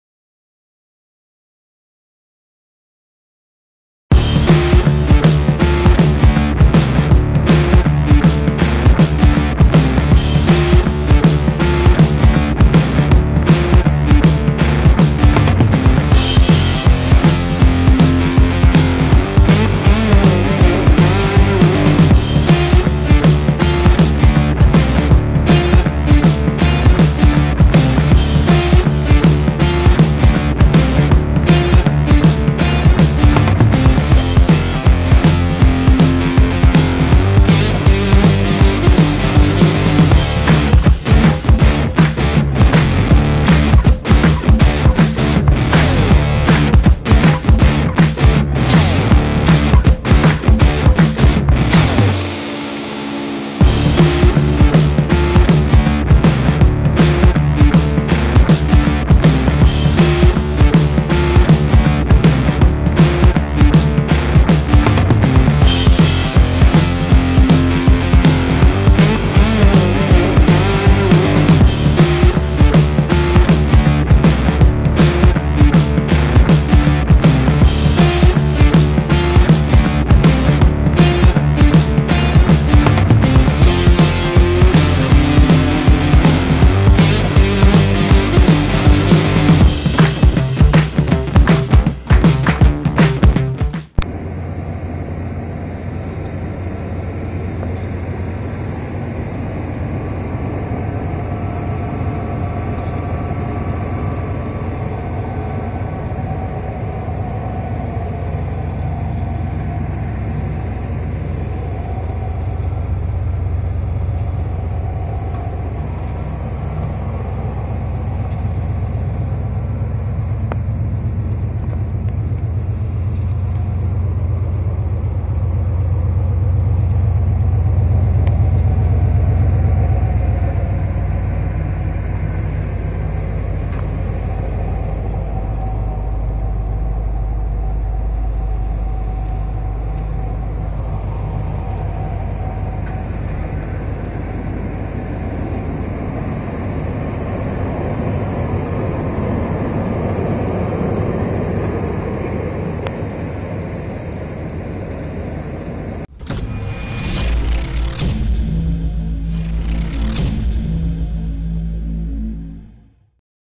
1996 Ford Bronco XL For Sale - Walk Around Video (176K Miles)
1996 Ford Bronco XL For Sale - Walk Around Video (176K Miles)
This 1996 Ford Bronco XL walk around is brought to you by GR Auto Gallery, complete with underside of the Bronco, as well as start up and running footage of the 1996 Ford Bronco XL.